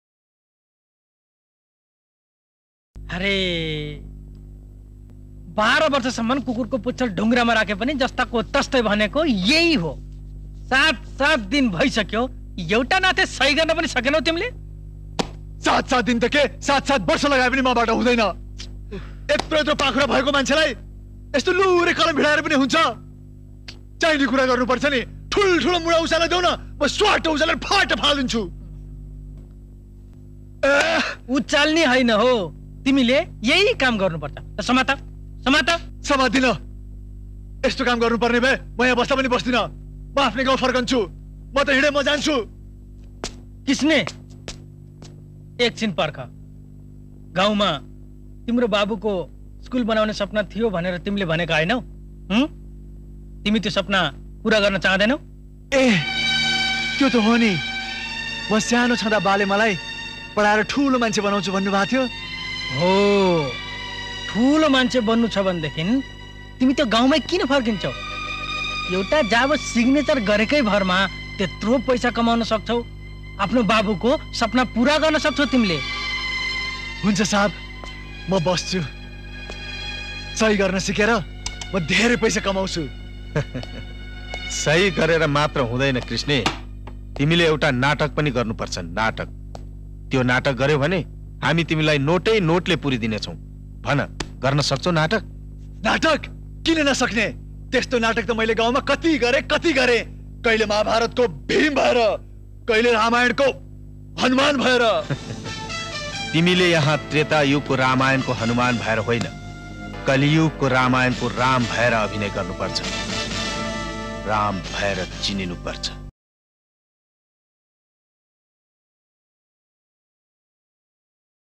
MR.KRISHNE SHORTMOVIE ||FT.Rajesh Hamal as Manish Bhurtel || Subash Shumsher Gautam || Anish Bhurtel
The short movie is of Voice over Acting of Rajesh Hamal /Ashok Sharma acting as existing Nepali cinema Mr.Ram Krishne Directed by Ashok Sharma.
Learning about Dialogue Delivery and cinema and Remembering our senior actors is the best way to respect them their arts.